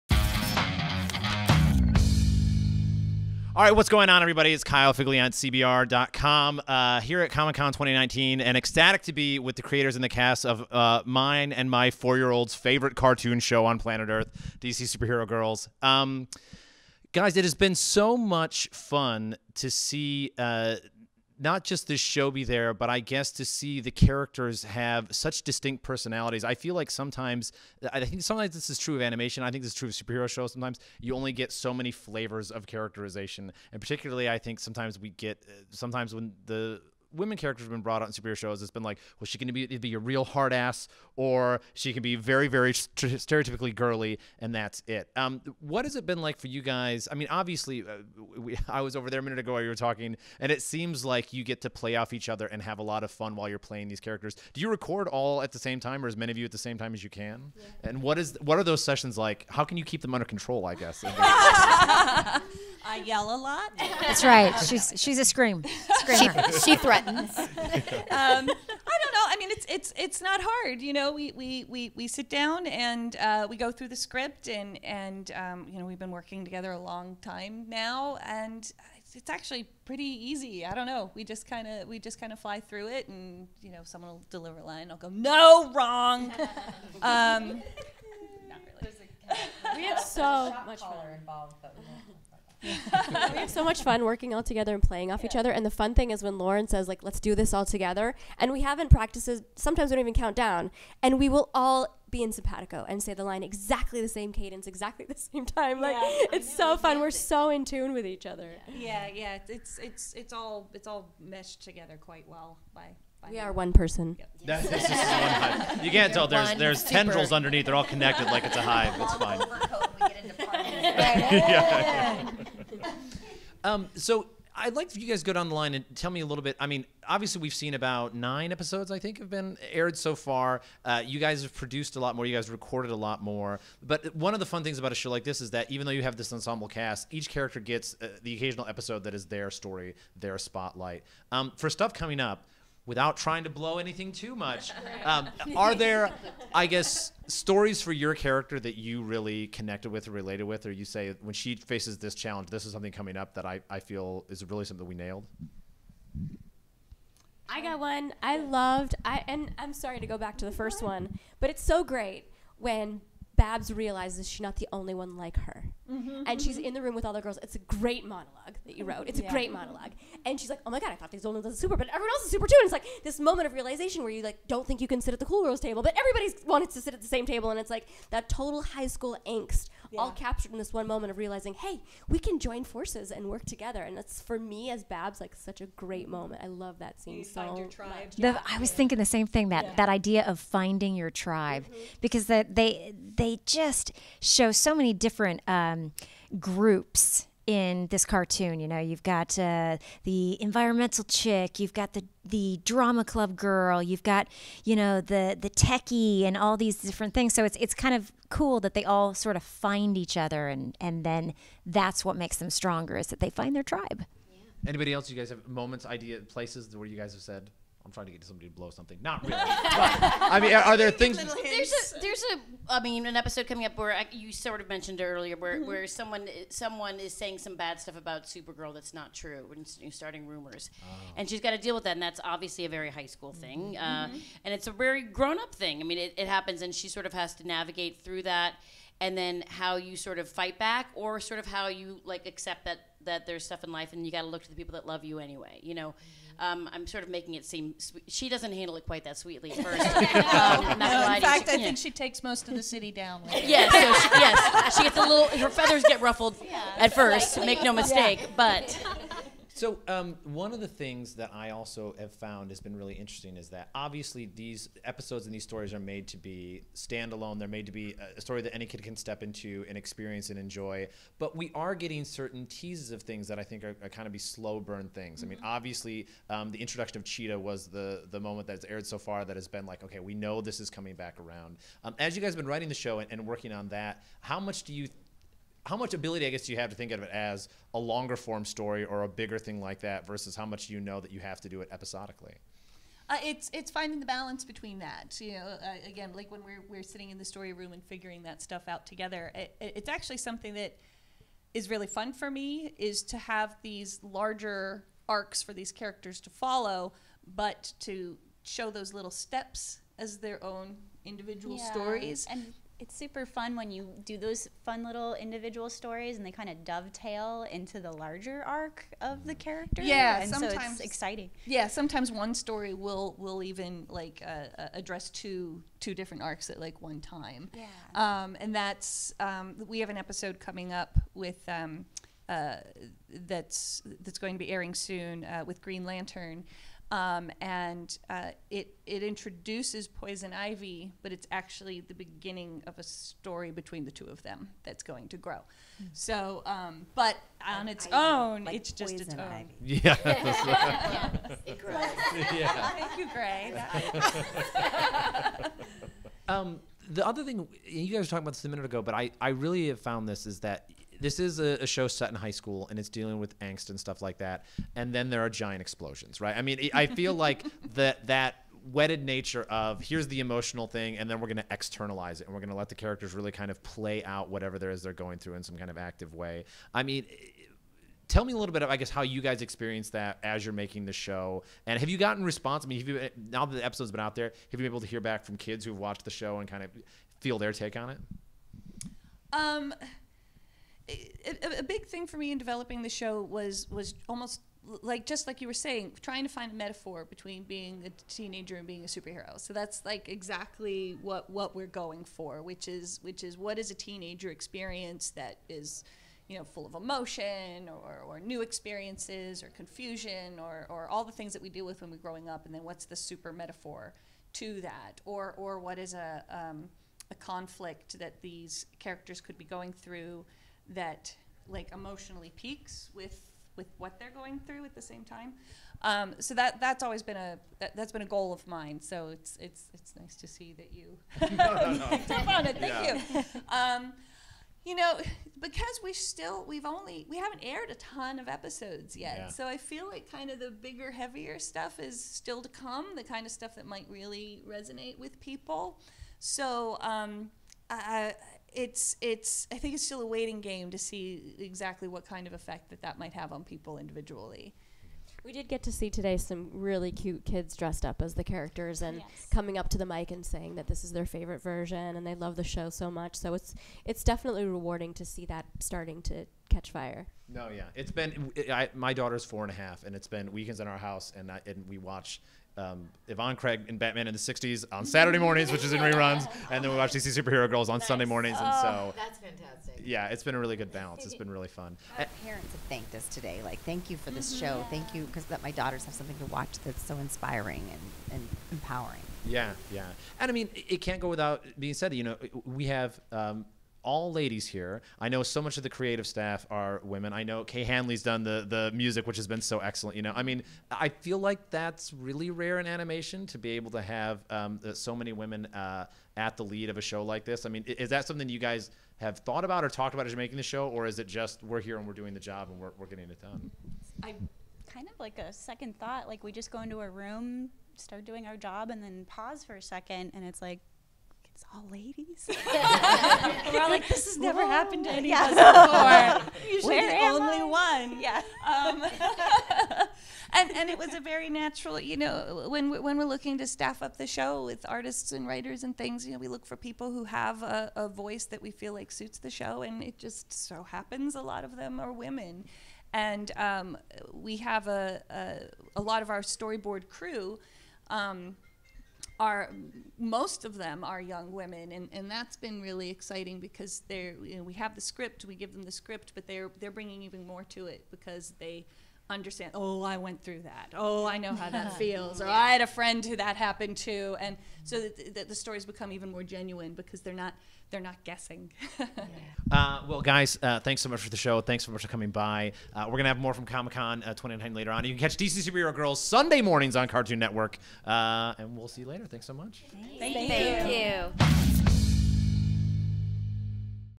DC SUPER HERO GIRLS INTERVIEW: Lauren Faust & DC Super Hero Girls Mix Real Angst with High Action
Lauren Faust, Tara Strong and the rest of the DC Super Hero Girls explain how the show mixes real world problems with high-octane fun and action.